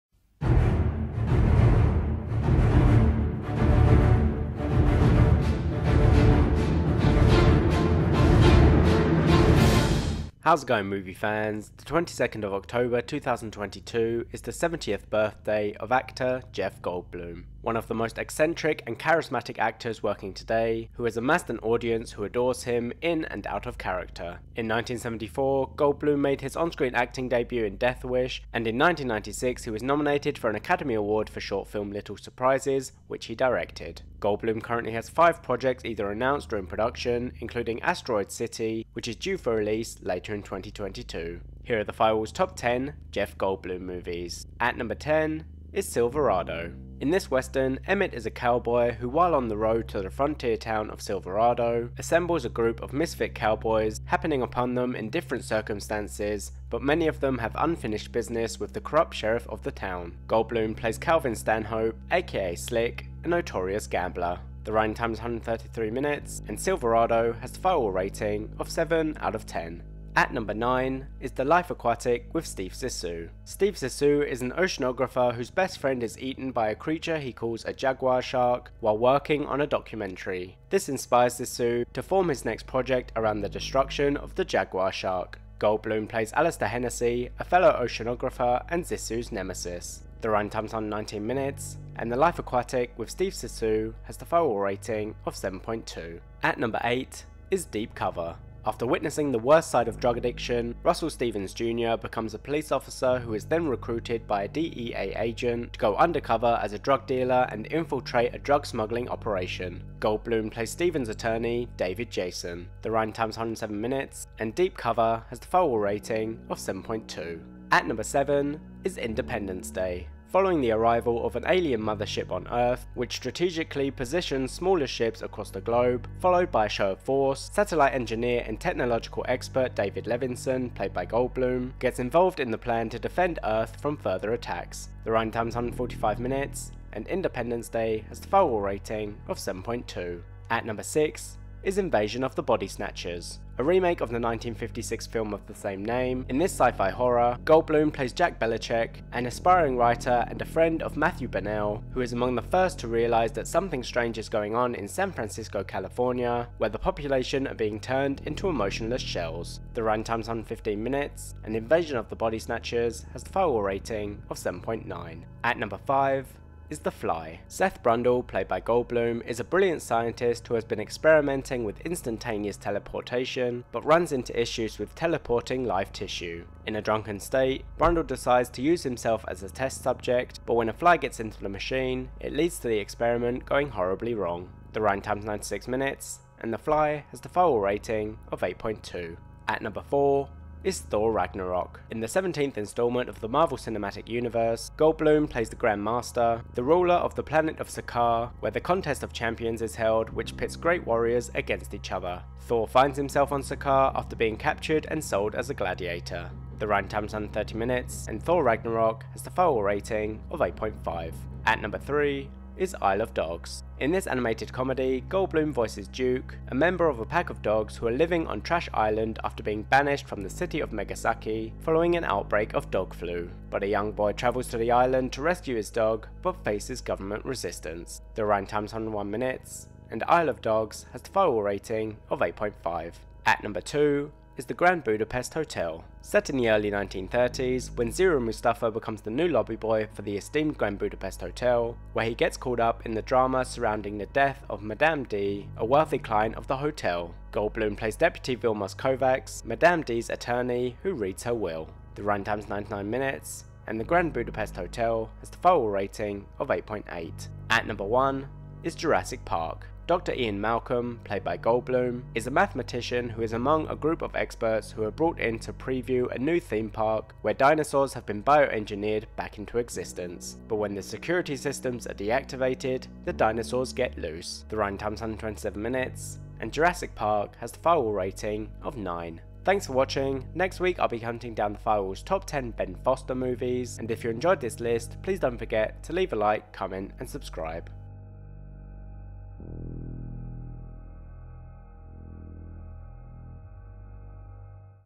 Top 10 Jeff Goldblum Movies
Today (22nd October 2022) is the 70th birthday of actor, Jeff Goldblum. One of the most charasmatic and eccentric actors working today, who has amassed an audience who adores him in and out of character. In 1974, Goldblum made his on-screen acting debut in Death Wish and in 1996 he was nominated for an Academy Award for short film Little Surprises, which he directed. Here are The Firewall's Top 10 Jeff Goldblum Movies.